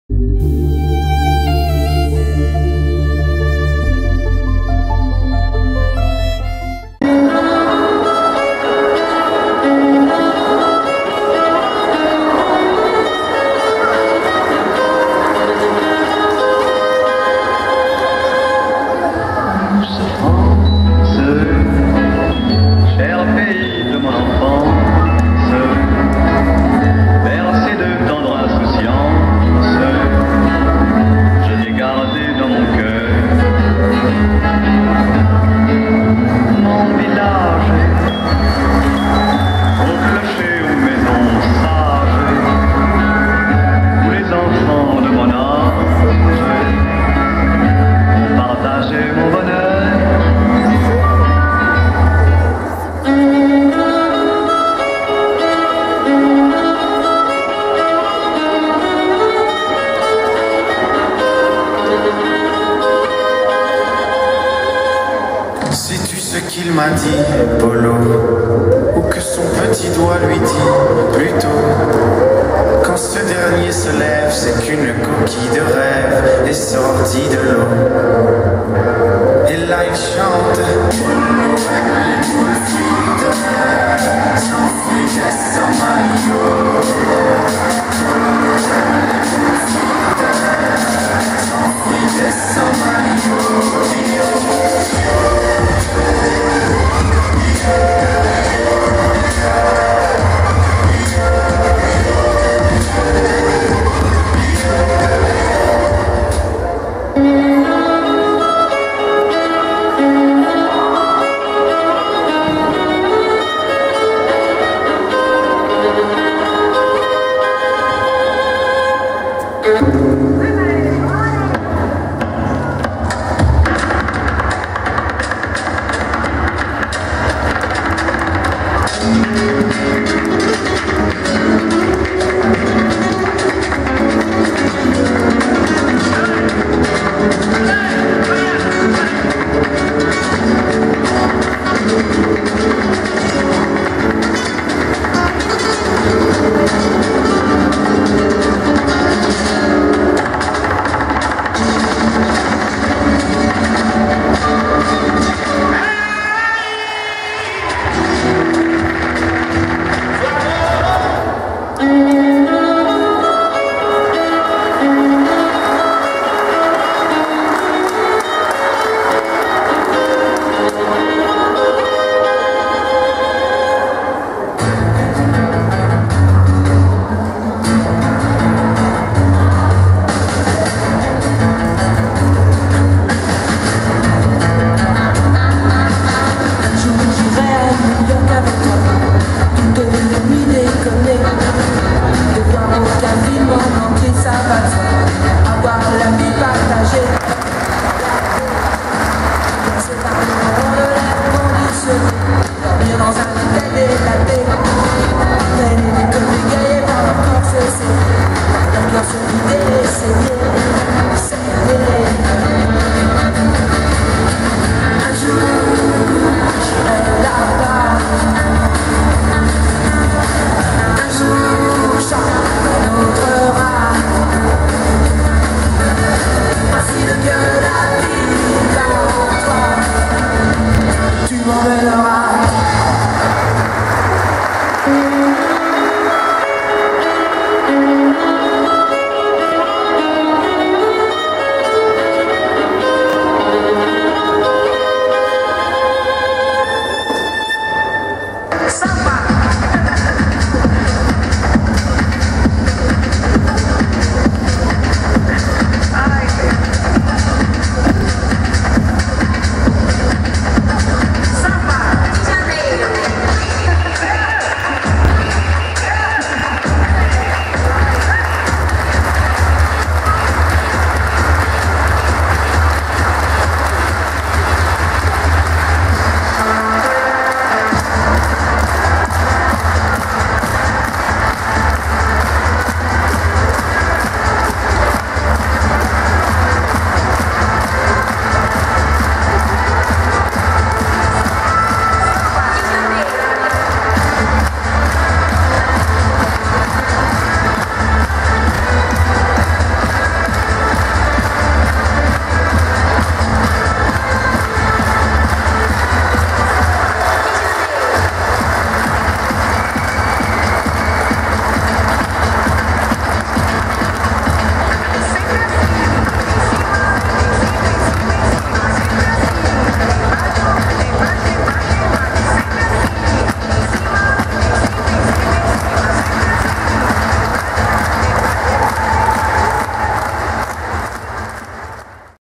acrovoyage par le collège Paul Bert Auxerre
Acrosport – Rencontre UNSS Yonne 2015 :
Chorégraphie présentée par les associations sportives scolaires des établissements de l’Yonne.